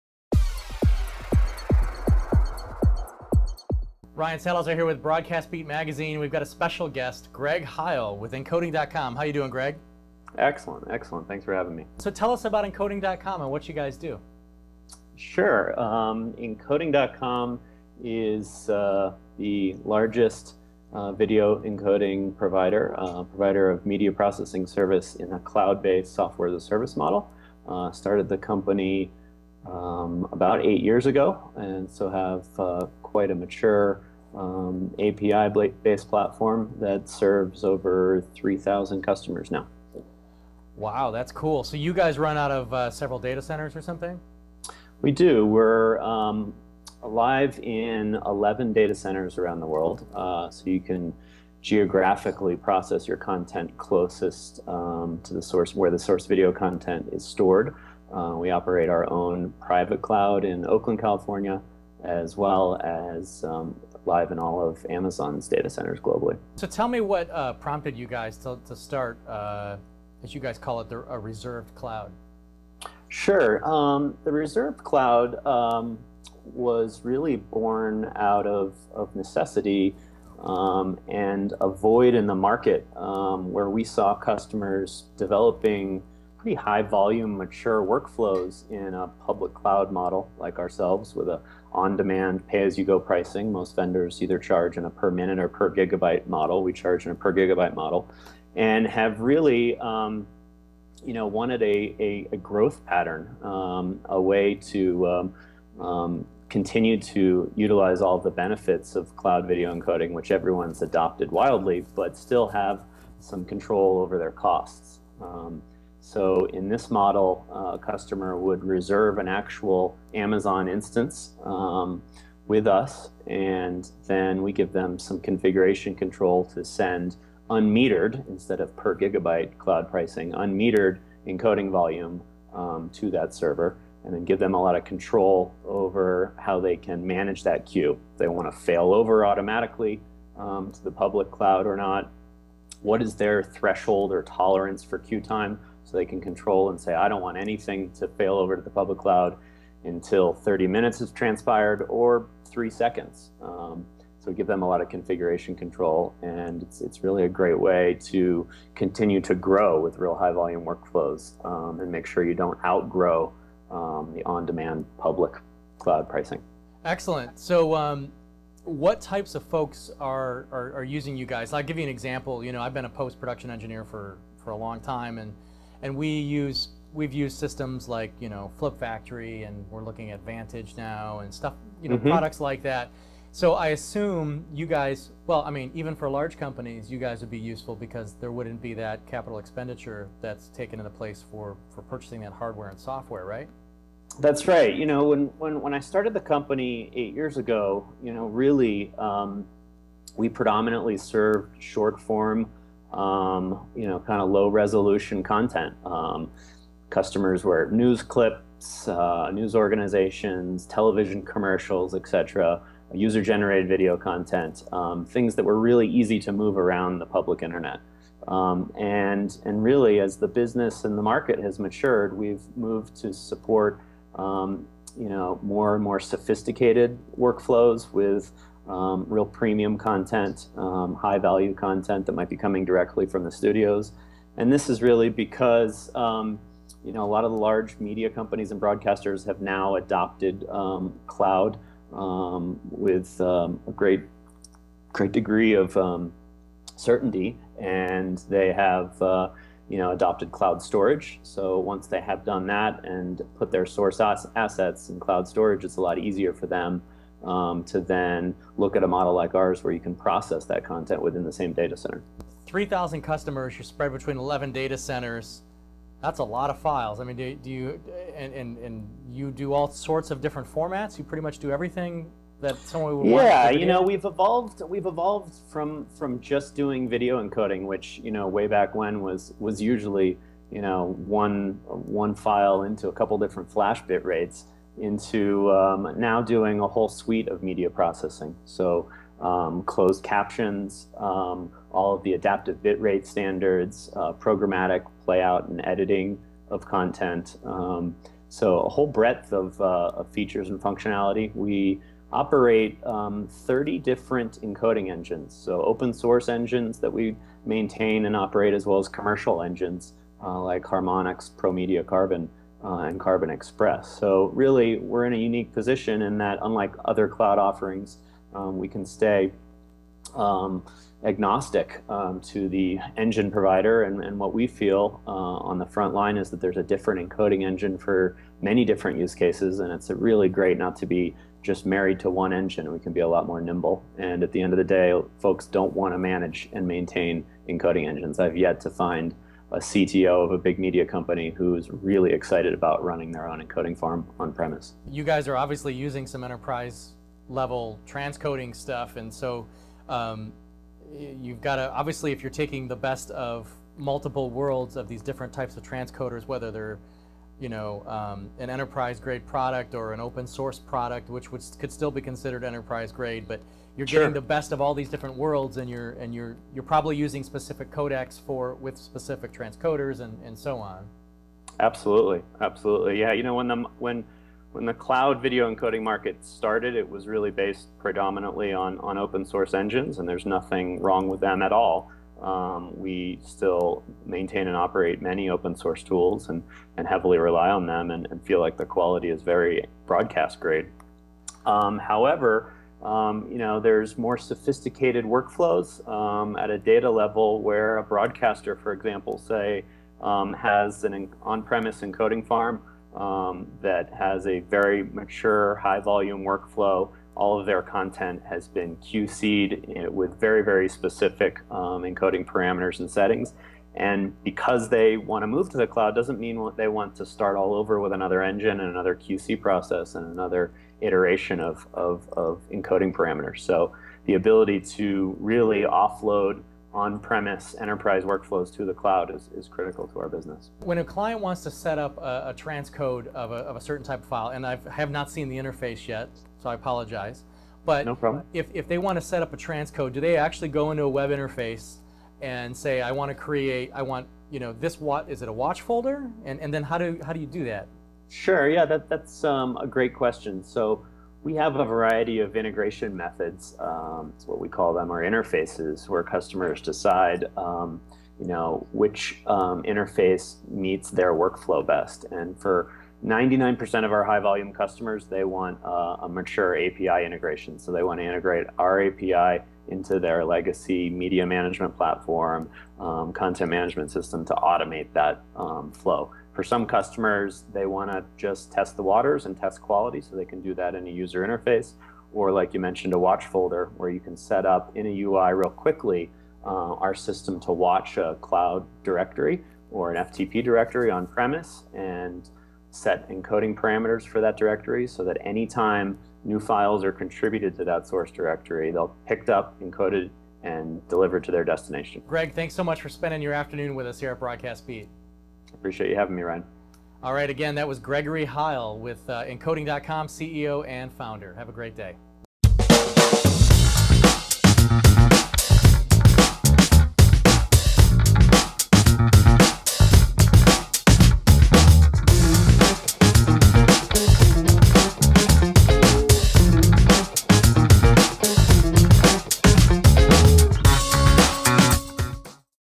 Encoding.com Talks Transcoding with Broadcast Beat
Greggory Heil, CEO and Founder of Encoding.com chats with Ryan Salazar of Broadcast Beat Magazine. Gregg touches bases on how their cloud offering works and how different types of organizations use transcoding for production processes.

Gregg also talks about Encoding.com’s suite of over 30 open source and commercial transcoding engines that cover a broad set of professional media processing requirements. All modern web, mobile, OTT, broadcast and edit codecs, adaptive bitrate packaging, closed captions, DRM encryption, and programmatic editing features are supported. All engines are maintained and updated remotely by Encoding.com. Clients no longer need to manage software licenses or worry about patches, hotfixes, or updates.